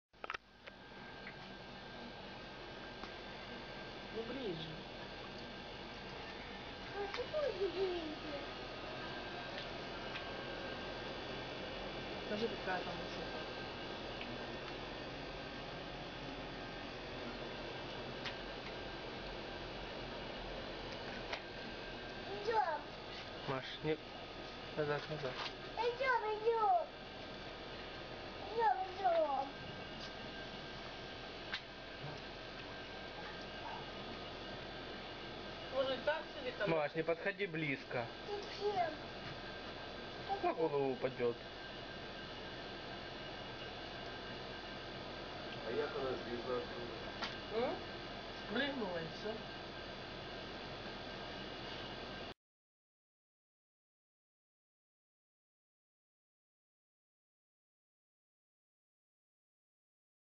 Turtle Climbing Up The Wall
The turtle is OK. It didn't fell on the ground.